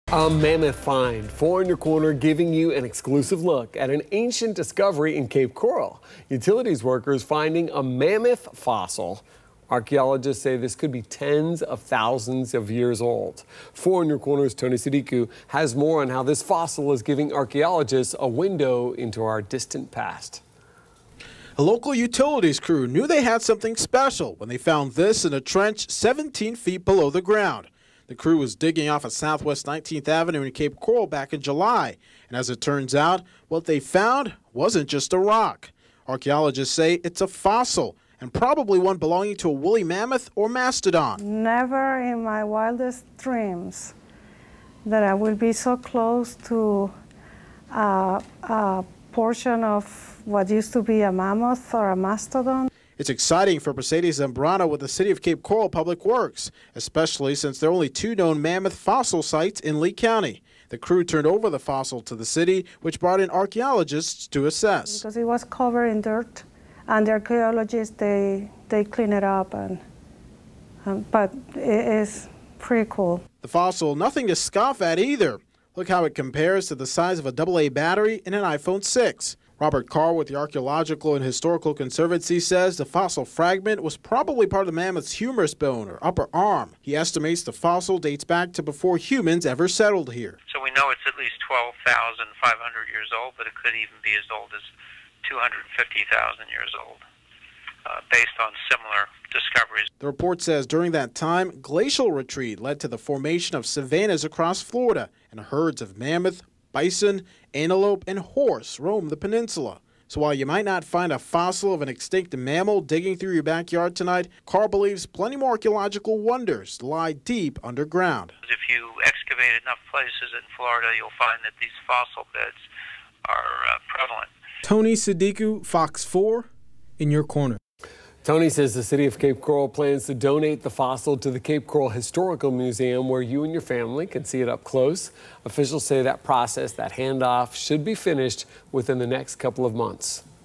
First hand look at mammoth bone in Cape Coral
Woolly mammoths in Cape Coral? Archaeologists believe a fossil found in Cape Coral could be tens of thousands of years old.